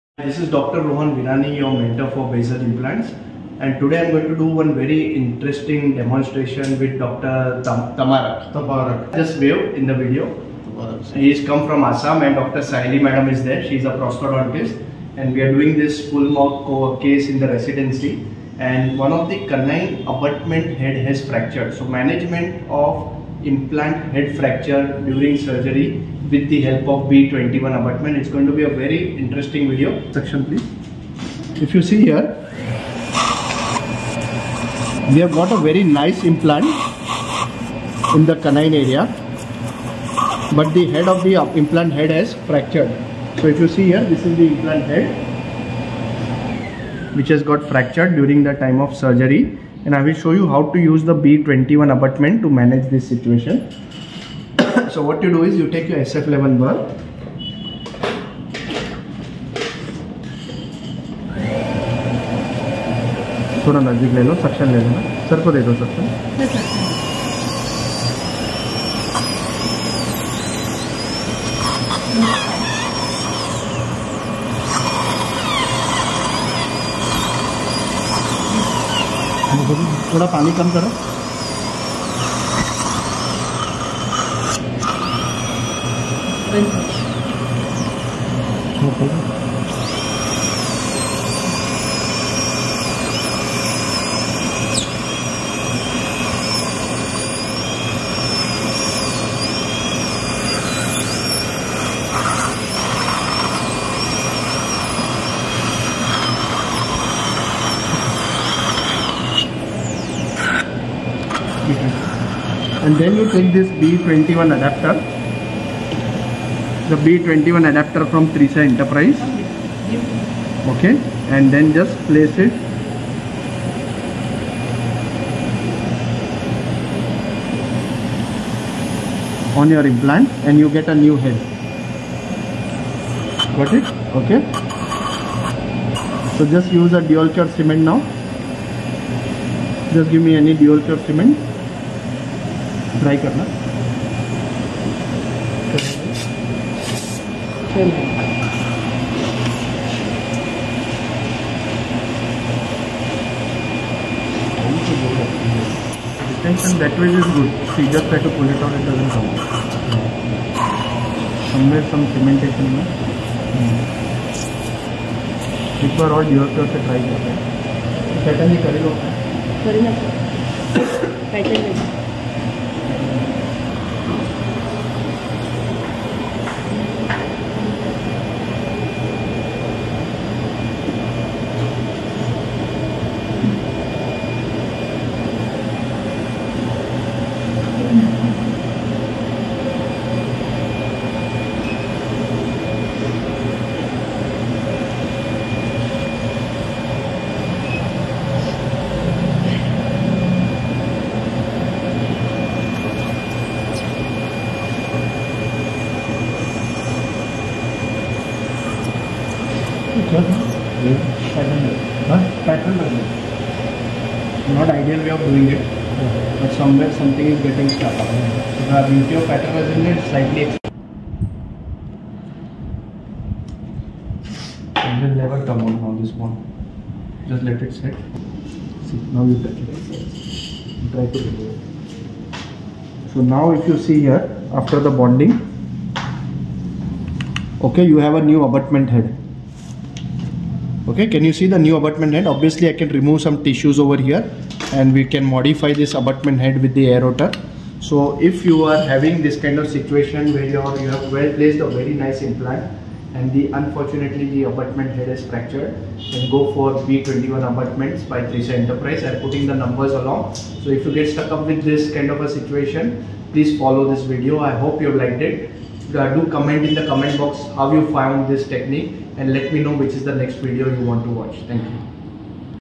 HOW TO USE B21 ABUTMENT IN BASAL IMPLANTOLOGY - EXPLAINED BY DR ROHAN VIRANI
To get daily new videos and updates from Dr Rohan Virani

Address :  1st Floor, Bhagyashree Apartments, B/3, Dr Ambedkar Rd, near Bank of Baroda, Mulund West, Mumbai, Maharashtra 400080